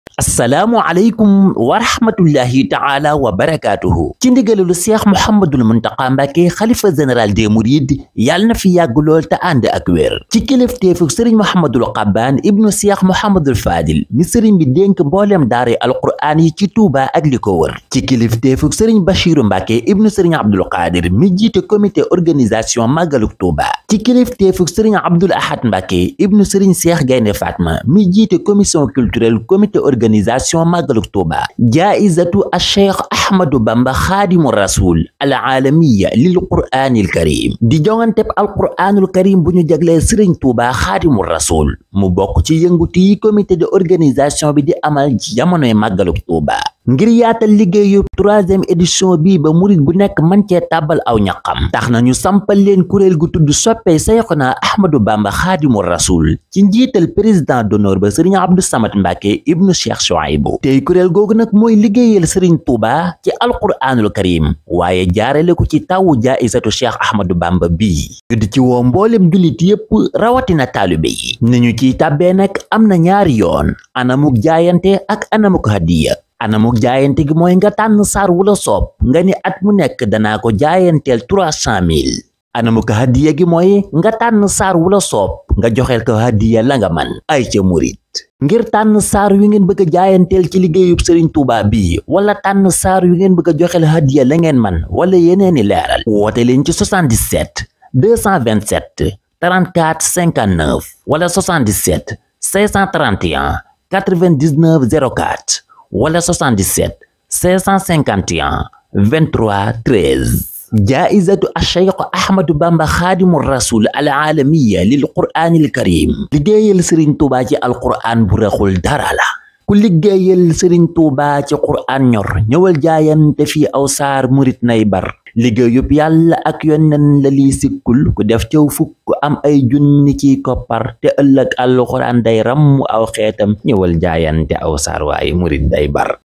Annonce grand Prix international Cheikh Ahmadou Bamba Khadimou Rassoul pour le saint coran 2024
Mourides.info S.A.R.L
Serigne Mbacké DIENG : Directeur de Publication